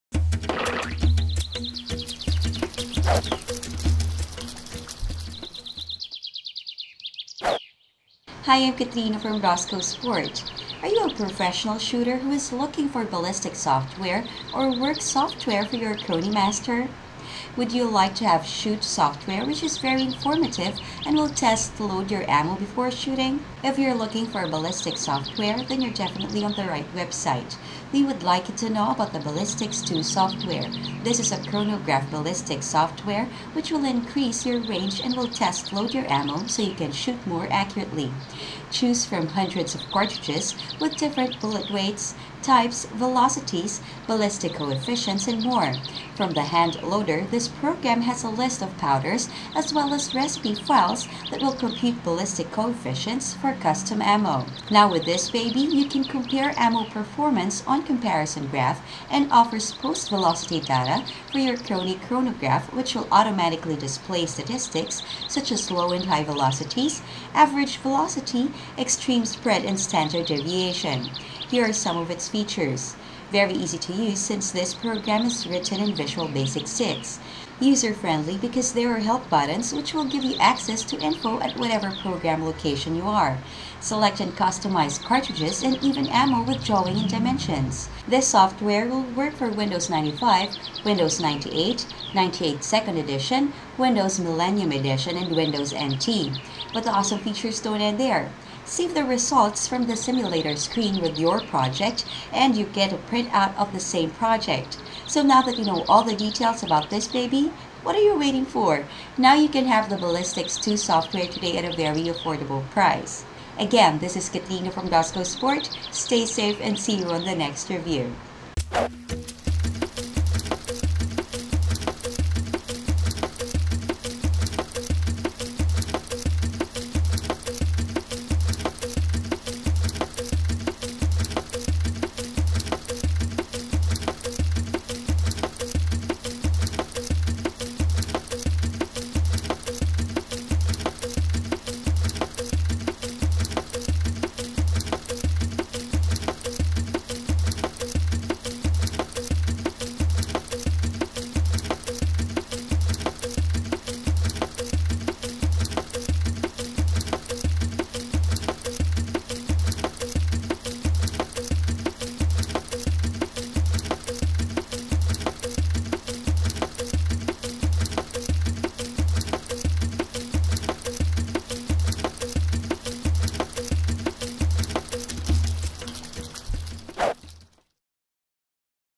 Chrony Ballistics II Software Review
It is Ballistics II Software; this is Chronograph Ballistics software which will increase your range and will test load your ammo so you can shoot more accurately.  Choose from hundreds of cartridges with different bullet weights, types, velocities, ballistic coefficients and more; for the hand loader this program has a list of powders as well as recipe files that will compute ballistic coefficients for custom ammo.  Now with Ballistics II Software you can compare ammo performance on comparison graph and offers post velocity data from your Chrony chronograph which will automatically display statistics such as Lo and Hi-velocities, average velocity, extreme spread and standard deviation.